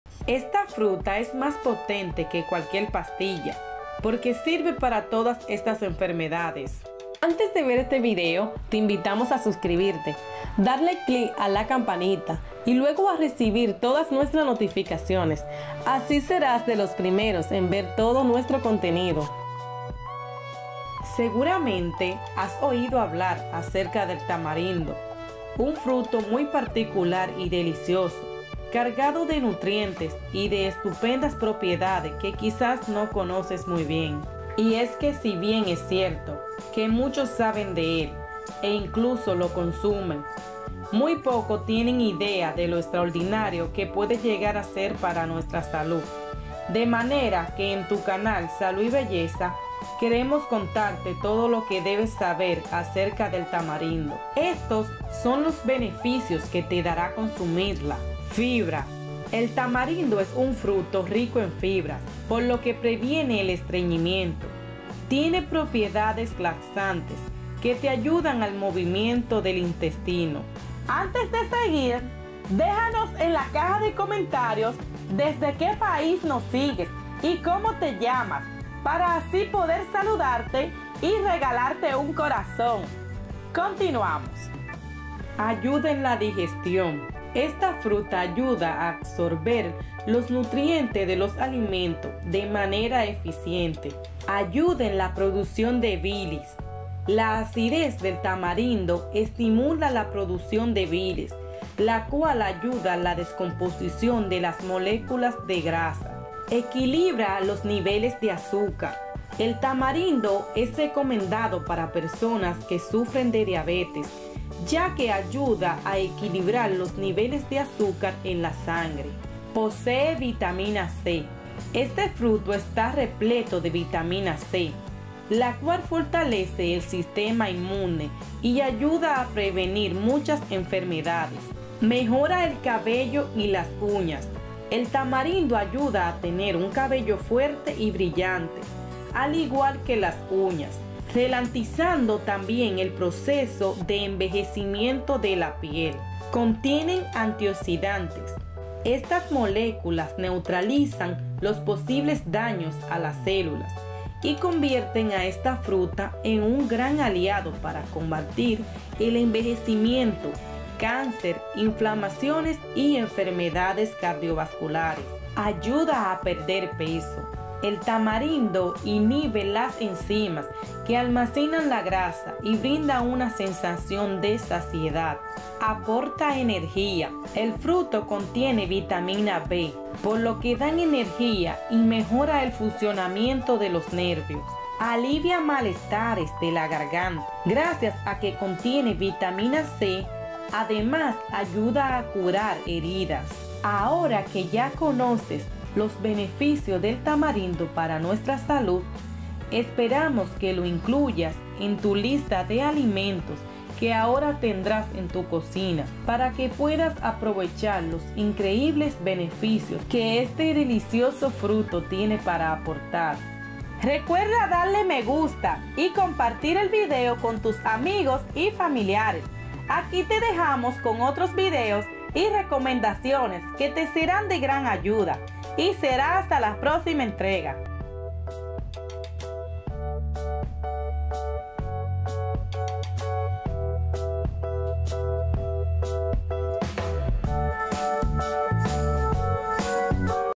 No + Pastillas! Si COMES esta Fruta no volverás a Sufrir de estas enfermedades
El tamarindo es una planta que tiene su origen en África. Su fruto es una vaina alargada que cuenta con una pulpa amarronada de sabor agridulce. En algunas regiones se ha venido utilizando para la elaboración de postres, bebidas y salsas. Pero no sólo se emplea en la cocina, también para curar distintas enfermedades.

Estos son los beneficios que te dará consumirla...

Compártelo así aportarás a la causa para que un amigo o una persona que necesite se beneficie de esta información importante.